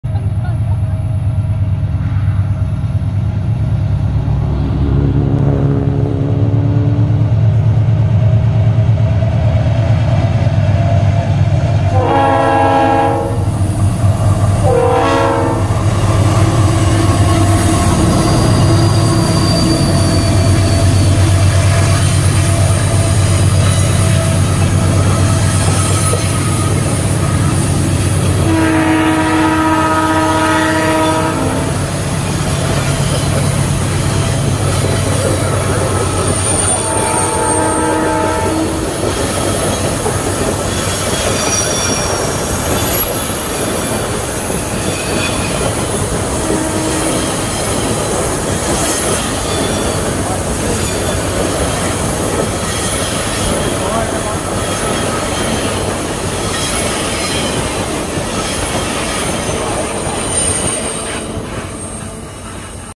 Trem passageiros da vale/luzes de natal
Lindo trem passageiros saindo do túnel.